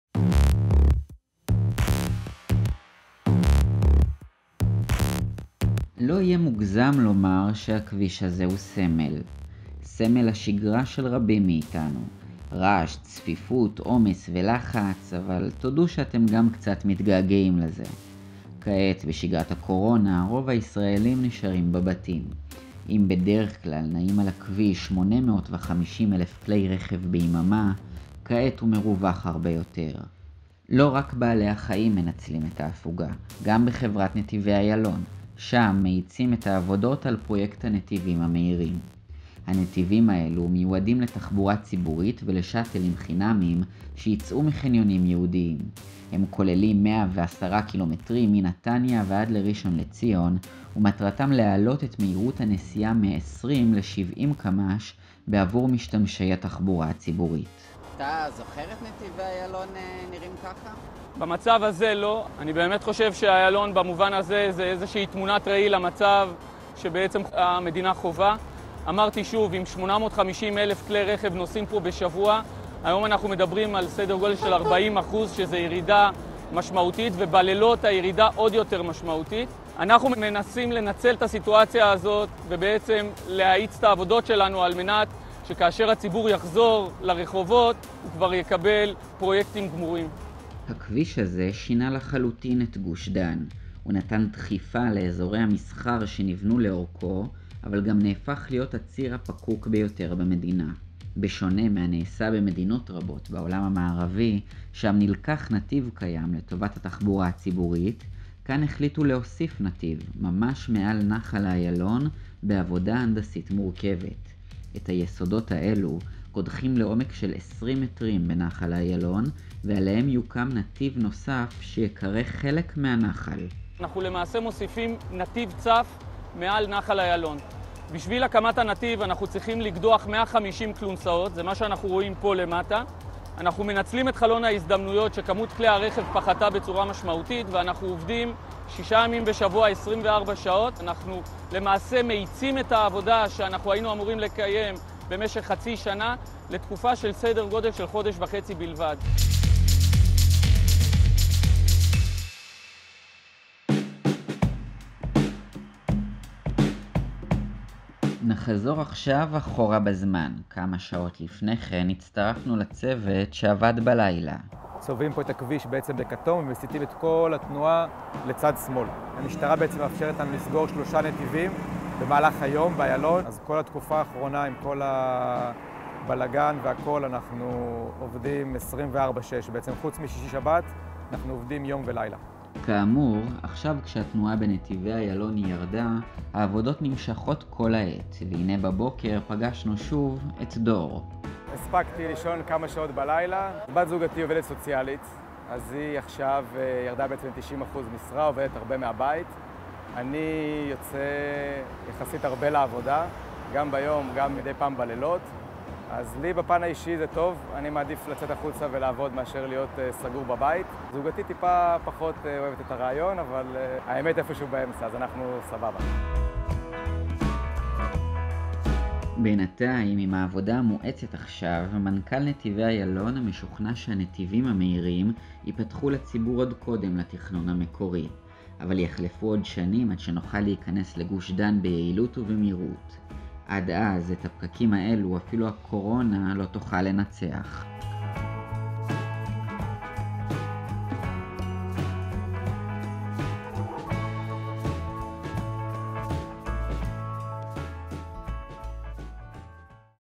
פחות מכוניות, יותר עבודות: כך מנצלים את הקורונה בכבישים
הגבלות הקורונה השאירו מאות אלפי רכבים בחנייה וב"נתיבי איילון" החליטו לנצל את התקופה להאצת העבודה על הנתיבים המהירים. המטרה: קיצור הנסיעה בתחבורה ציבורית מראשון לציון ועד לנתניה. הצצה לעבודות מסביב לשעון